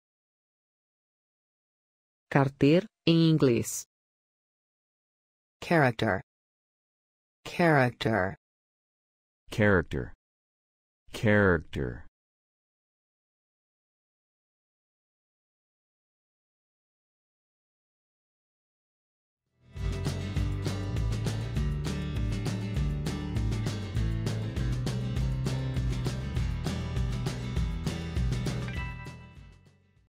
Como dizer carter em inglês
Como se diz corretamente carter em inglês? Ouça conosco.
Qual é a tradução correta da palavra carter do português para o inglês? Fale como os americanos. Além disso, como escrever corretamente a palavra character?

Obrigado!